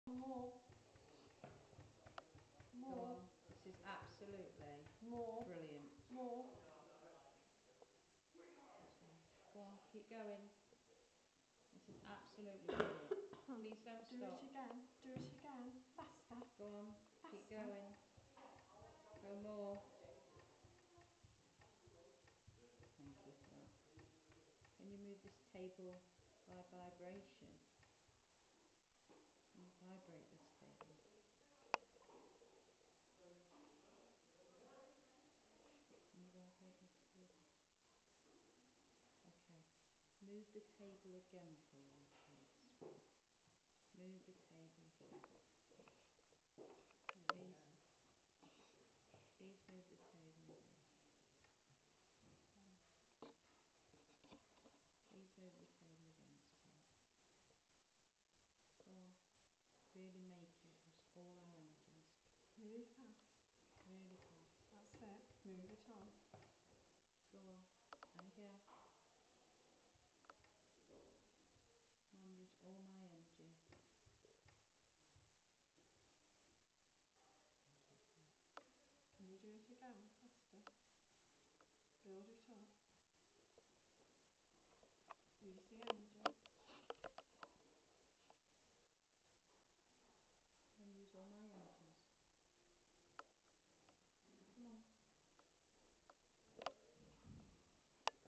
Table Tipping at Gainsborough Old Nick Theatre
Sorry about the audio again, it is best listened to through ear phones...

A large wooden table was rocking from side to side almost on command from the group.. None of the LGN Team members were touching the table at the time, it was left to the guests to build the energy for the spirits. Video taken in the auditorium.